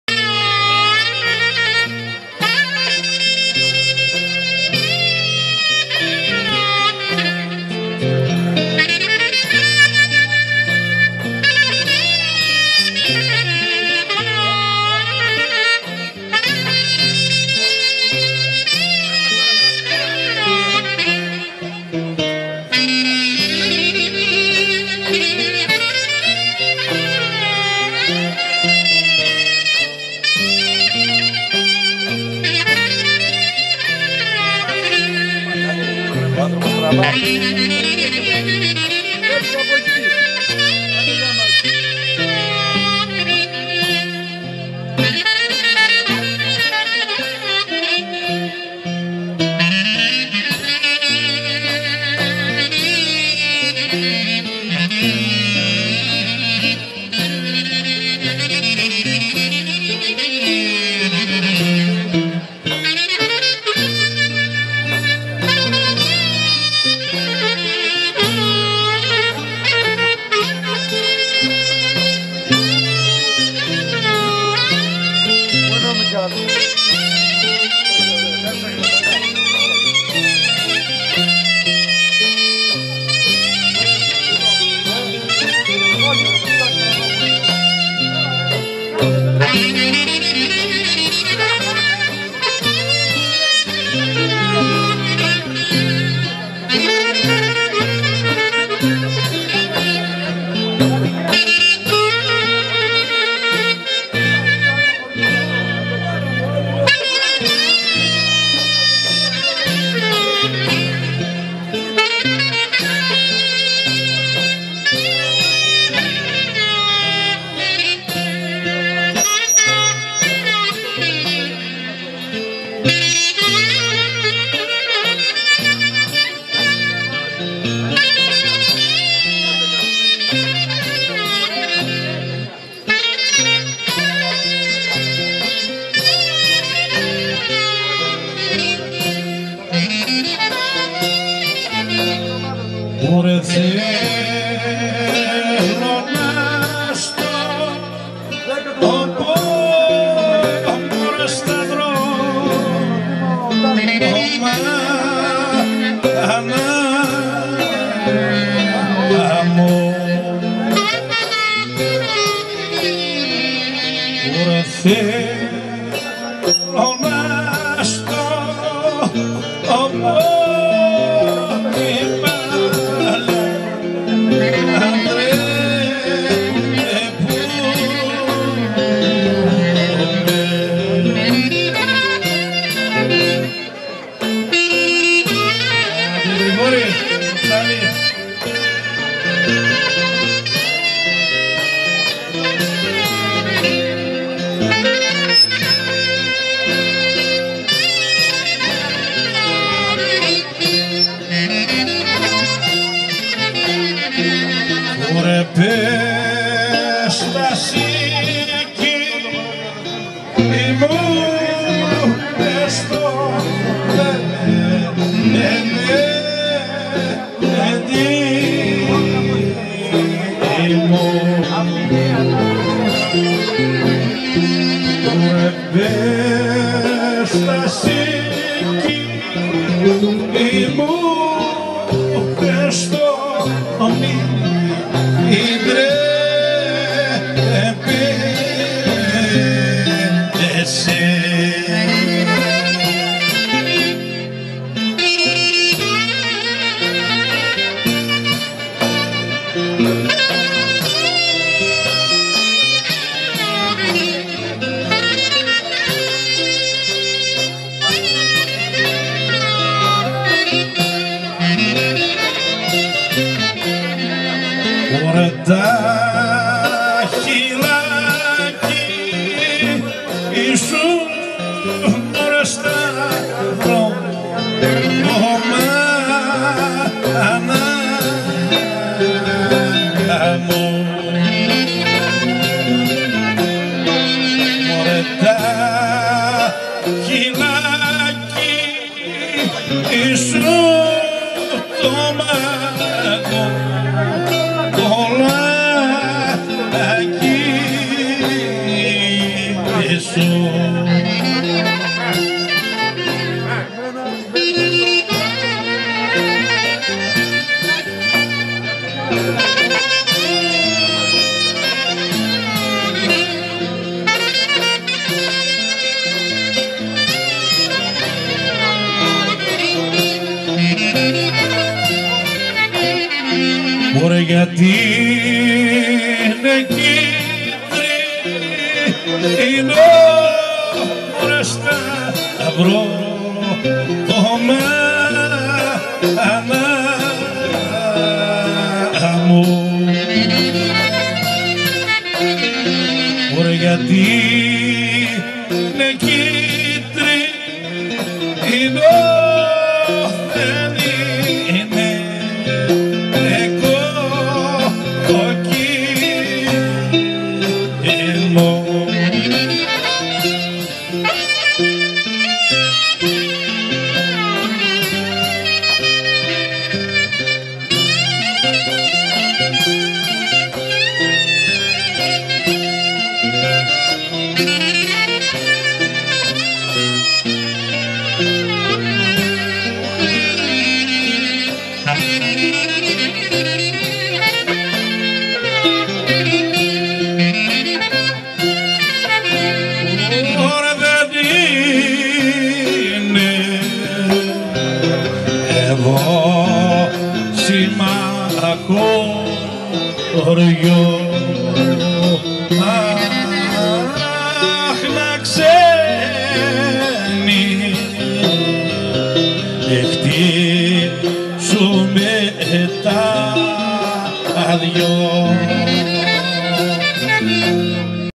Panigyri ston Elafotopo ton Avgousto tou 2007
Paradosiako panigyri ston Elafotopo Zagoriou ton Avgousto tou 2007 me tous Grigori kai Niko Kapsali sto klarino kai tin orhistra tous.Katapliktikes ekteleseis apo ton monadiko deksiotehni kai koryfaio tou eidous stin Zagorisia mousiki Grigori Kapsali.Mazi tou to megalytero klarino tis genias tou Nikos Kapsalis.